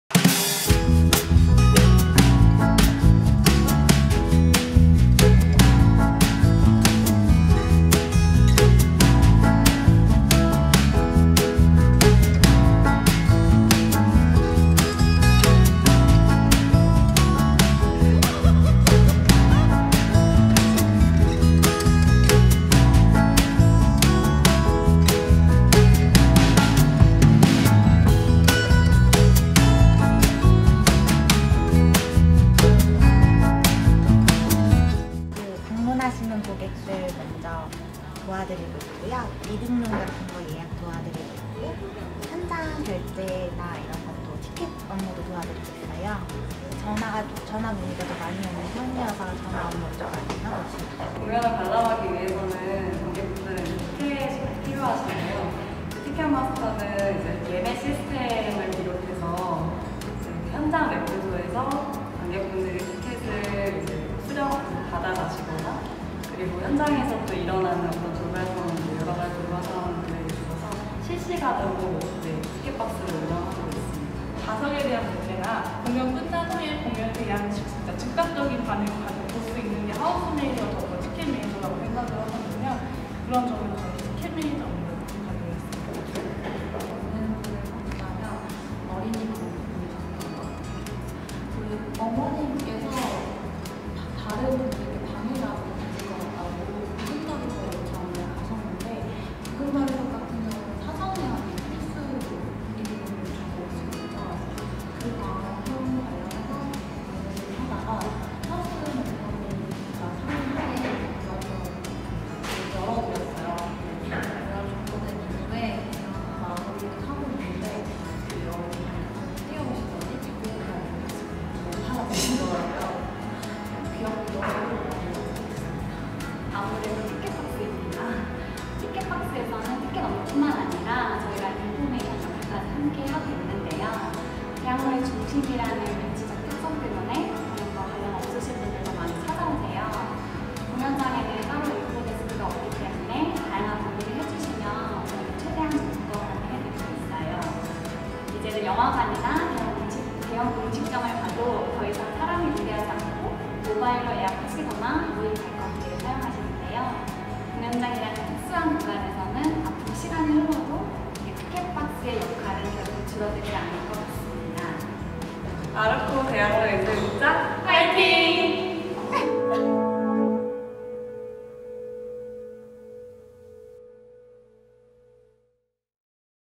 [아르코·대학로예술극장] 티켓마스터 인터뷰
공연장에 가면 항상 만날 수 있는 이분들. 어떤 업무를 담당할까요?
공연을 만드는 사람들 9편

티켓의 예약 상황과 발권, 배부를 책임지는 티켓마스터를 만나보겠습니다!